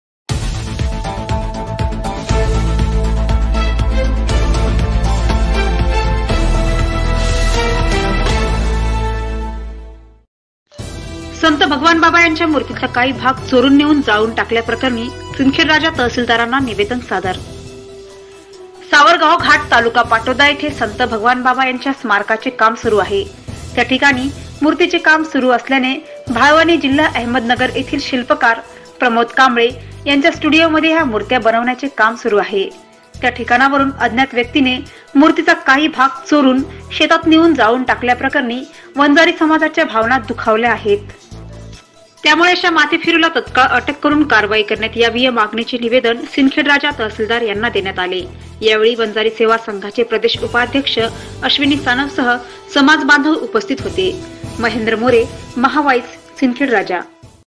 संत भगवानबाबा यांच्या मूर्तीचा काही भाग चोरून नेऊन जाऊन टाकल्याप्रकरणी...
सिंदखेडराजा तहसीलदारांना निवेदन सादर.

Ankar..सावरगाव घाट तालुका पाटोदा येथे संत भगवान बाबा यांच्या स्मारकाचे काम सुरू आहे त्या ठिकाणी मूर्तीचे काम सुरू असल्याने भाळवणी जिल्हा अहमदनगर येथील शिल्पकार प्रमोद कांबळे यांच्या स्टुडिओमध्ये ह्या मूर्ती बनवण्याचे काम सुरू आहे त्या ठिकाणावरुन अज्ञात व्यक्तीने मूर्तीचा काही भाग चोरून शेतात नेऊन जाळून टाकल्याप्रकरणी वंजारी समाजाच्या भावना दुखावल्या आहेत त्यामुळे अशा माथेफिरू ला तात्काळ अटक करून कारवाई करण्यात यावी या मागणीचे निवेदन सिंदखेडराजा तहसीलदार यांना देण्यात आले यावेळी वंजारी सेवा संघाचे प्रदेश उपाध्यक्ष अश्विनी सानप सह समाज बांधव उपस्थित होते. महेंद्र मोरे महा व्हाईस सिंदखेडराजा.